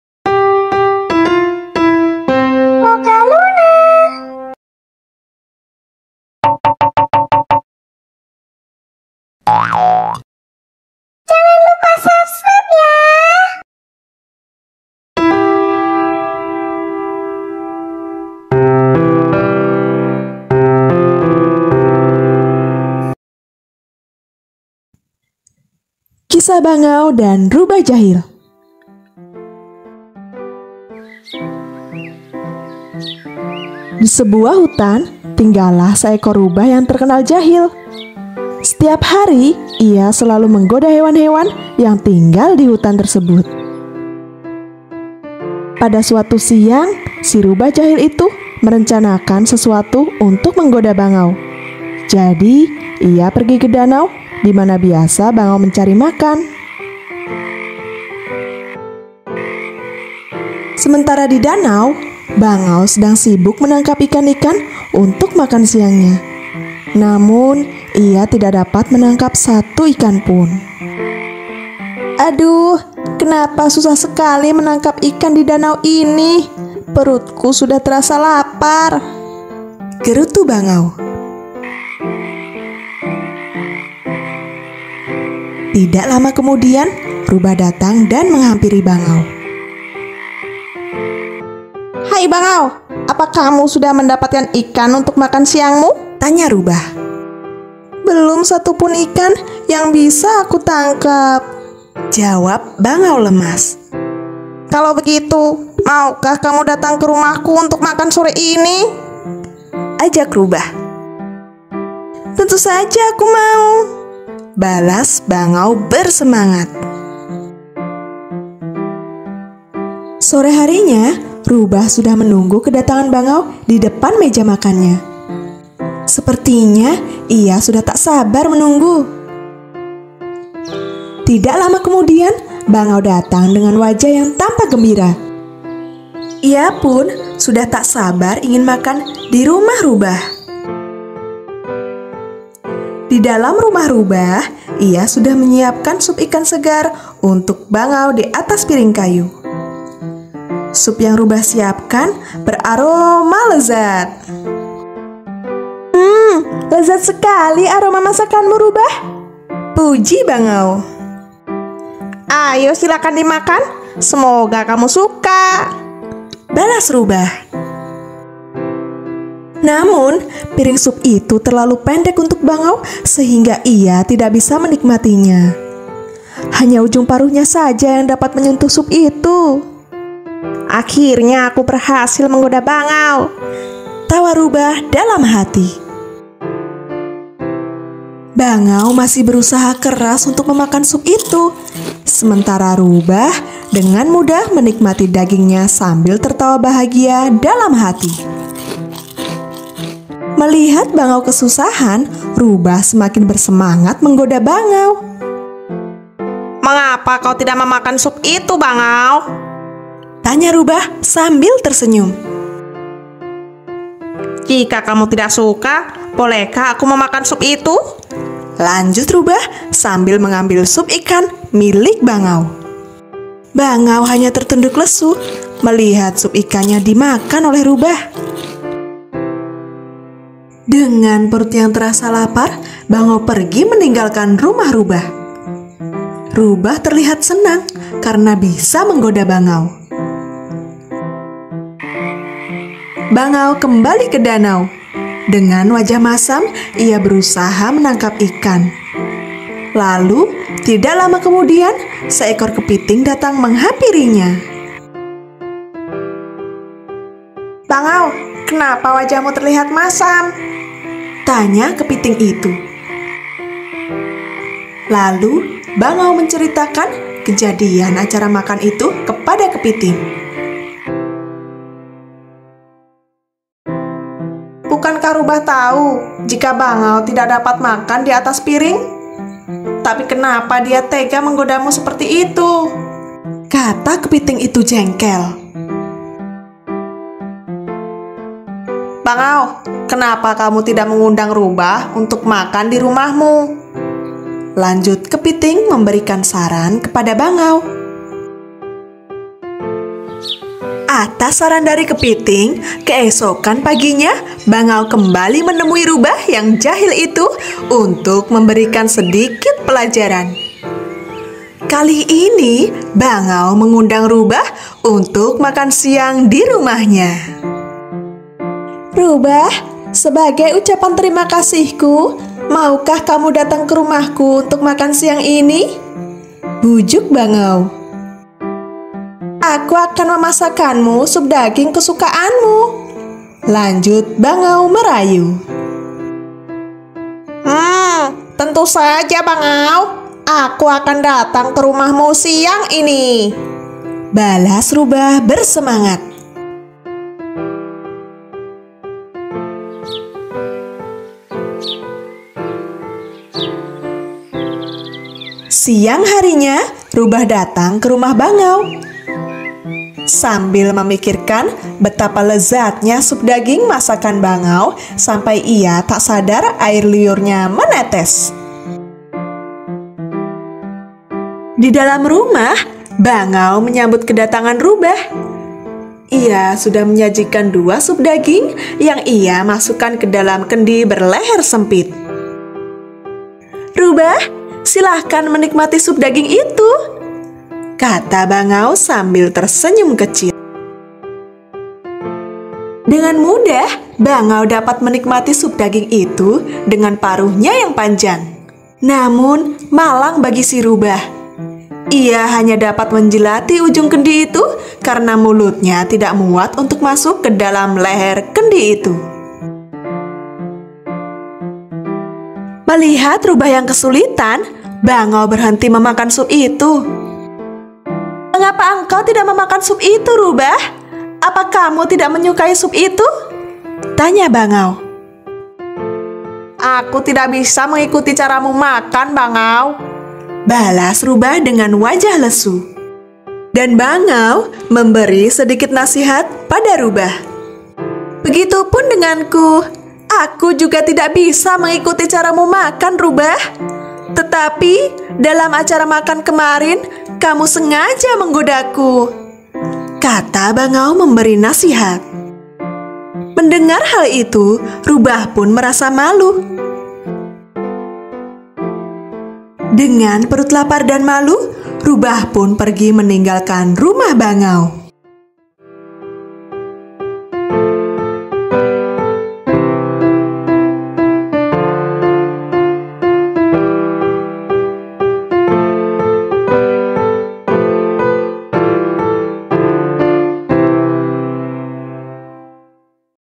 KISAH || BANGAU DAN RUBAH || CERITA ANAK BAHASA INDONESIA
KISAH BANGAU DAN RUBAH
halo kawan-kawan
kali ini mokaluna berkesempatan untuk membagikan kisah tentang bangau dan rubah yang jahil

terimakasih